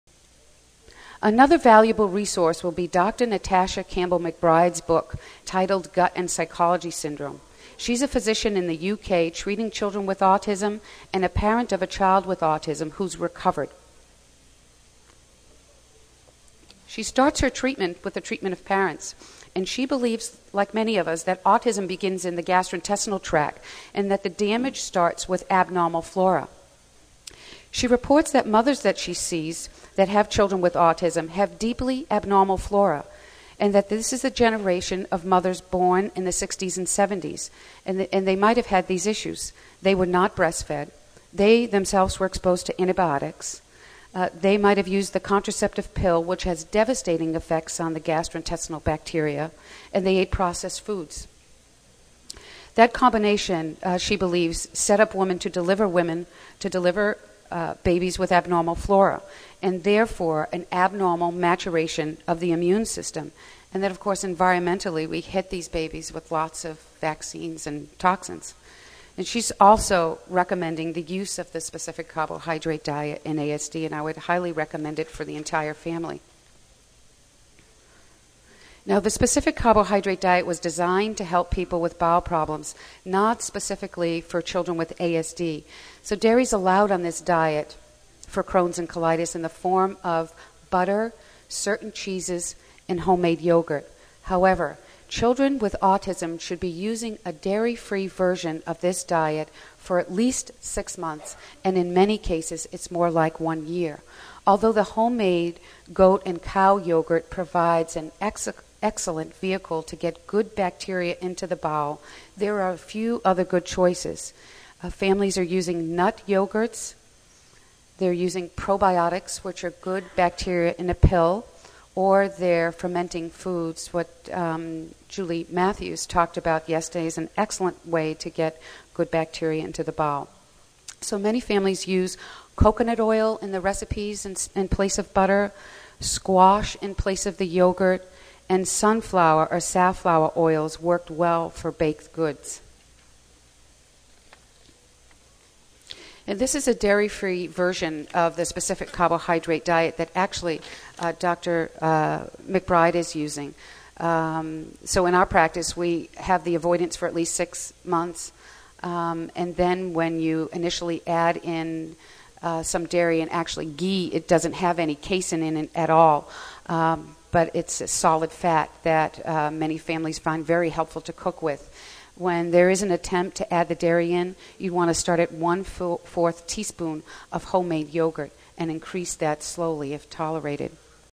Gastrointestinal Pathology and the Specific Carbohydrate Diet in ASD Part 6
Presented by Pamela Ferro, RN, ASN